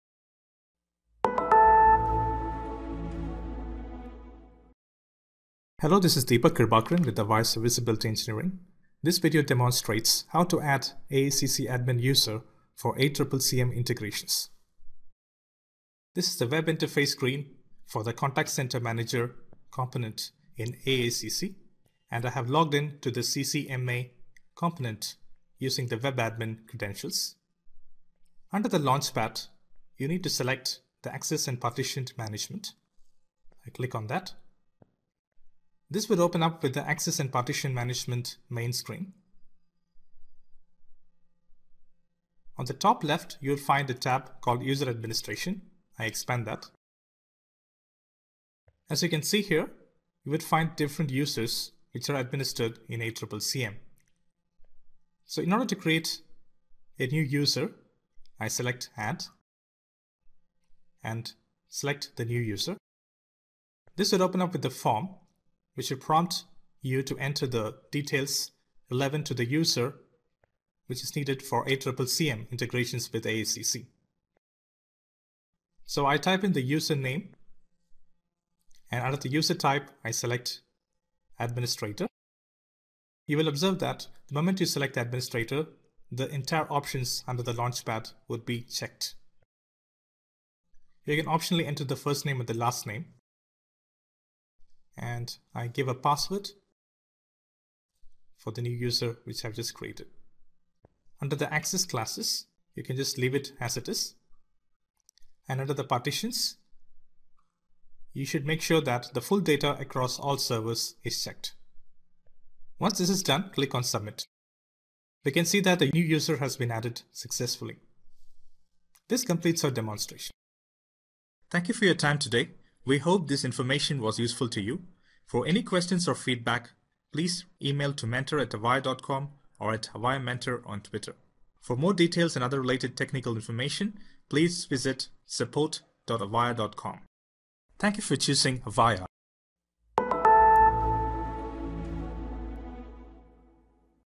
How to Add Admin User in AACC for Avaya Contact Center Control Manager Integration
This video demonstrates how to add Admin user in AACC for ACCCM integrations with a detailed demo. Produced by Deepak Kirubhakaran.